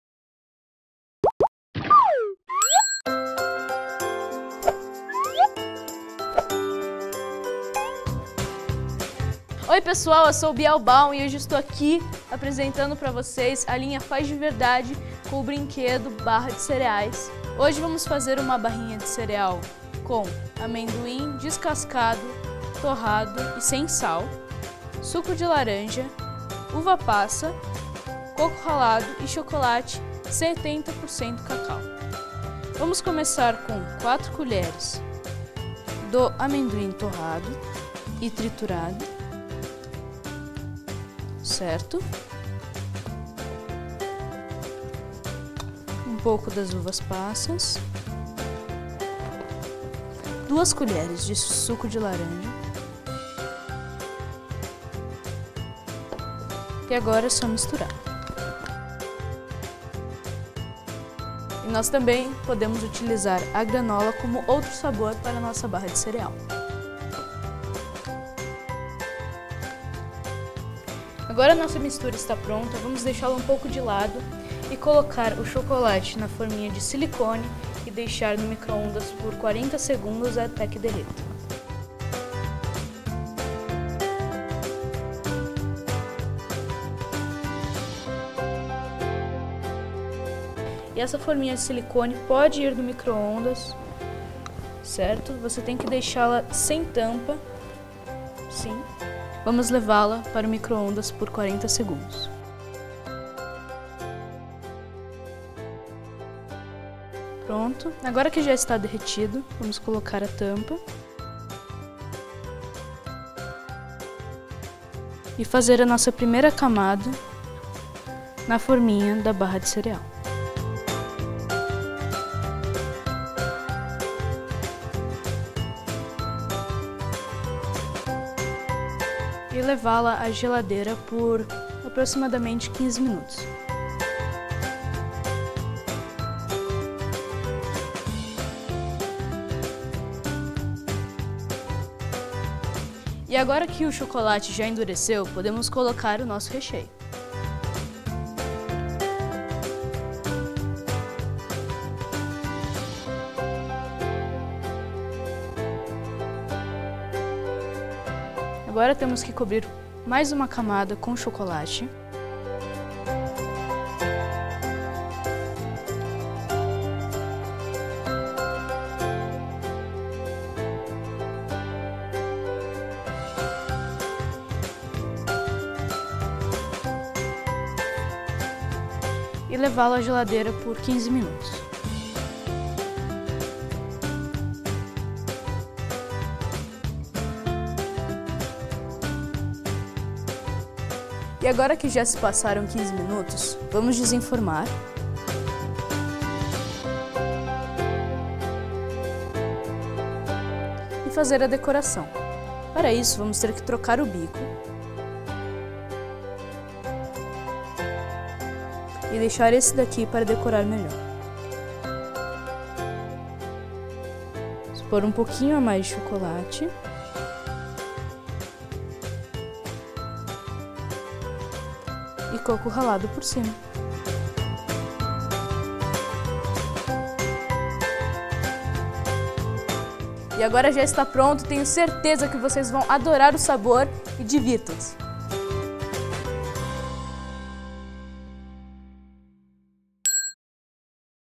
Manual em vídeo Fábrica de Barra de Cereais Receita deliciosa | Faz de Verdade | Brinquedos Estrela
Hora de brincar com a Fábrica de Barra de Cereais da Estrela! Aprenda com o chef mirim Biel Baum a fazer uma barrinha deliciosa e saudável com amendoim, suco de laranja, uvas passas, coco ralado e chocolate!